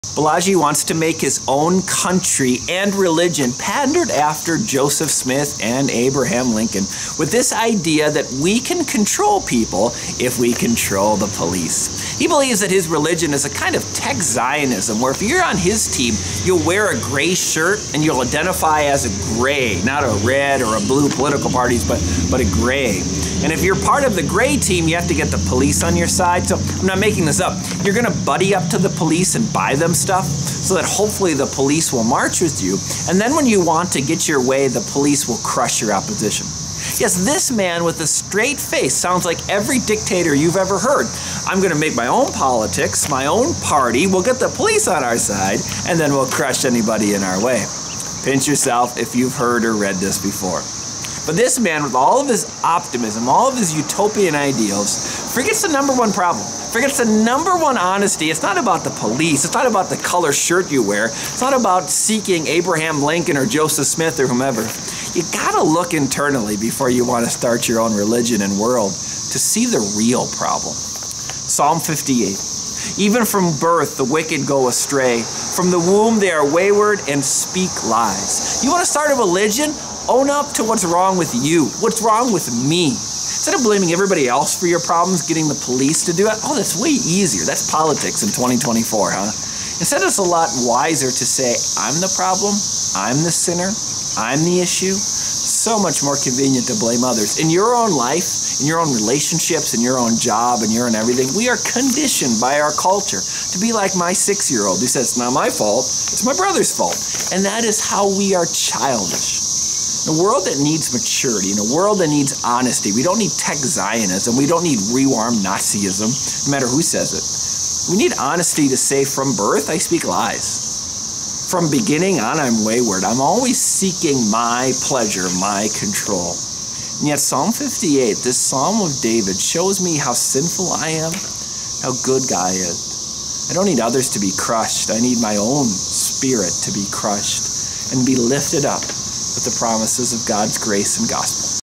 Billionaire Wants To Make "Tech Zionism" Of Grays
He wants his own country and religion, but promises to be good.
Where have we heard this before?